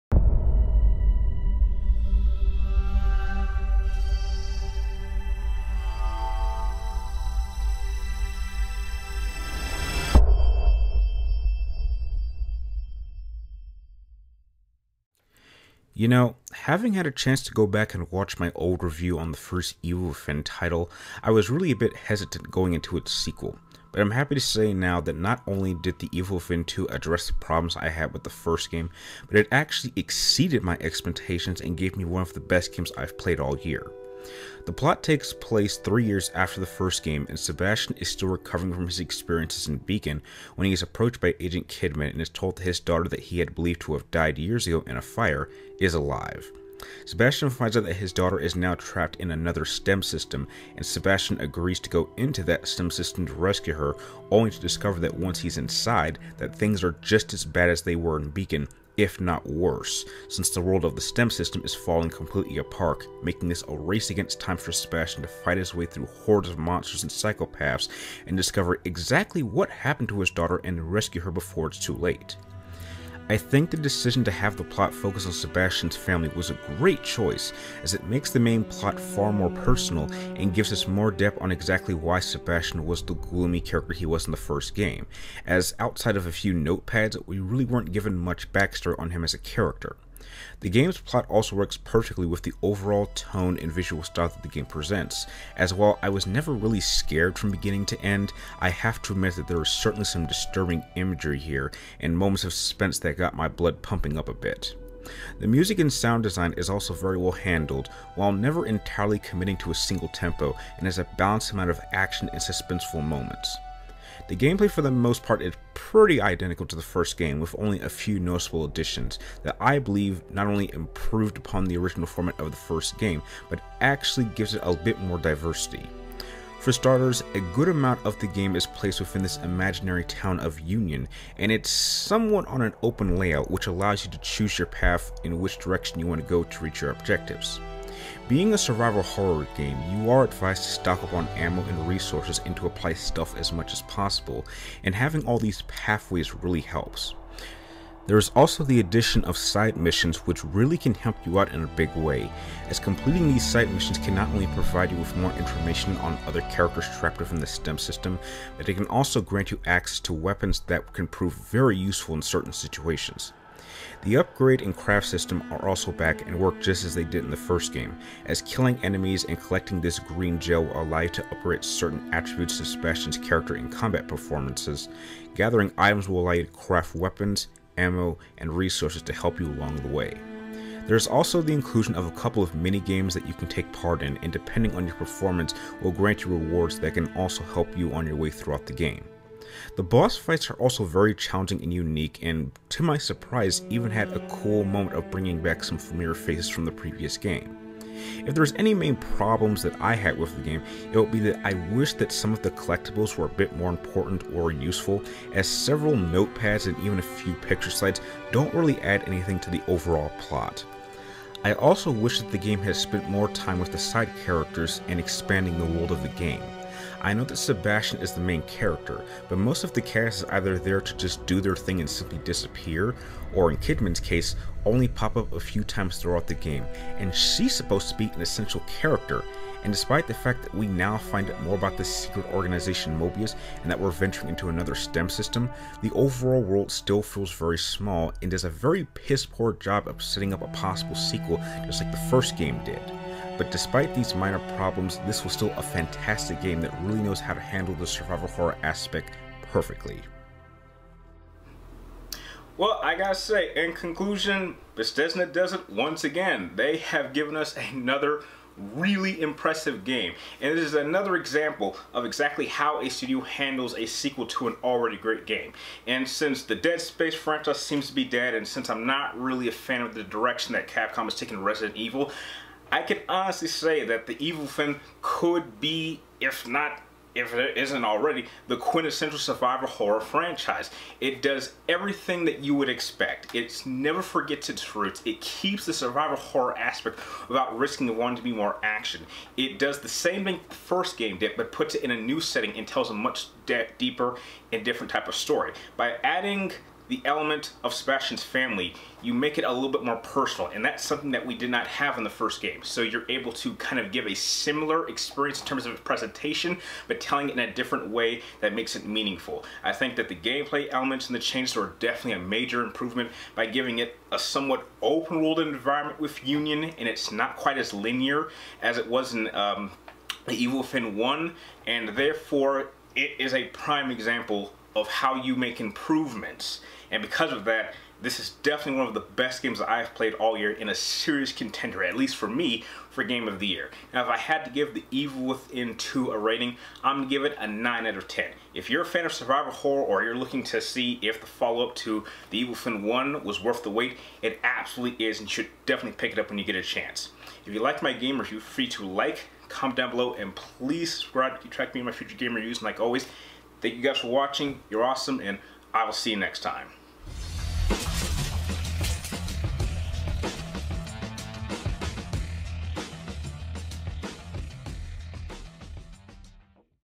The Evil Within 2 - Game Review | 3TR
Here is my review on the sequel to 2014s survival horror title that take things to a personal level. Does this new tone help enrich the story or simple come off as cheap.